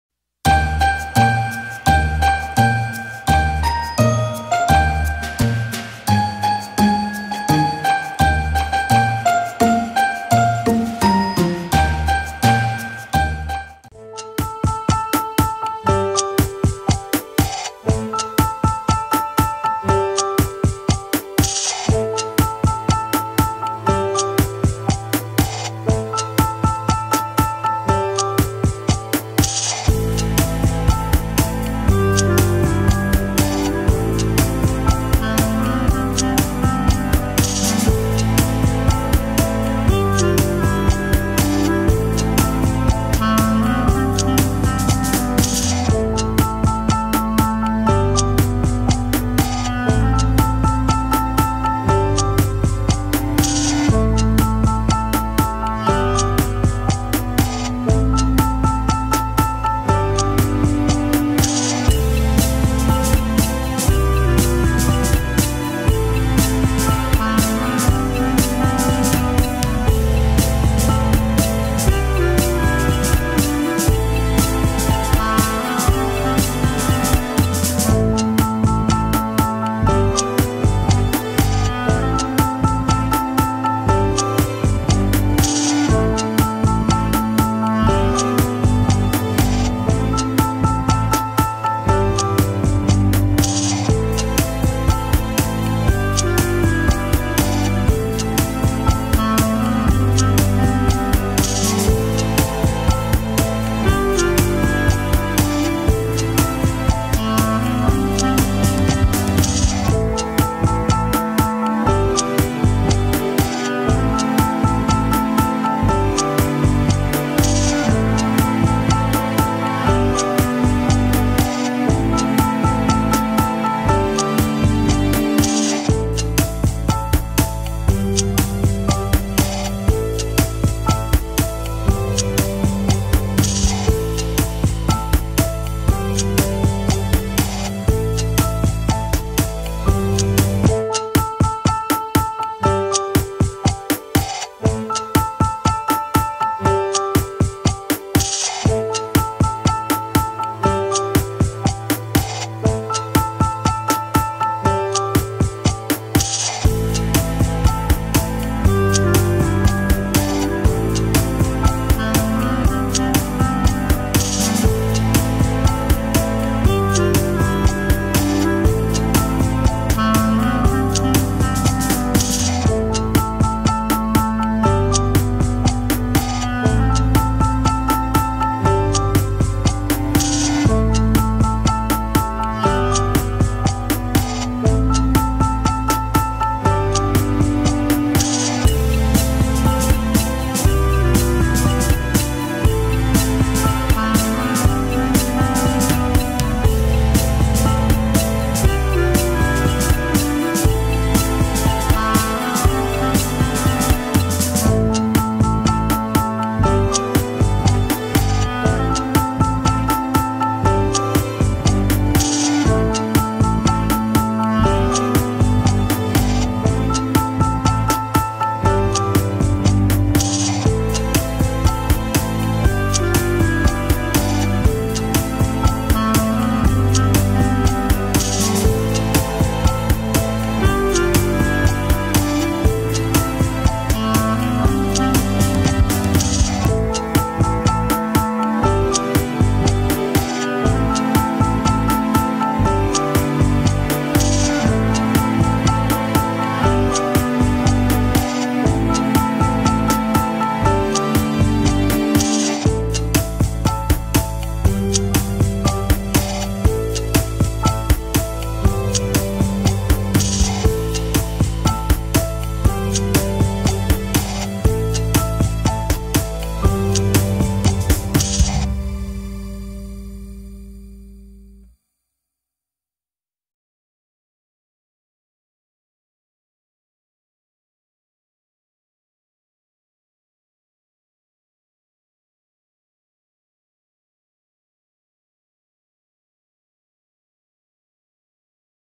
DIY: How to Make Glitter Foam Purse Key Ring - Easy 2 Do || Glitter Foam Sheet Crafts Idea
Welcome to my glitter foam sheet crafts youtube channel.
Watch how to make glitter foam mini purse with key holder step by step.  diy christmas crafts ornament making from eva foam sheets.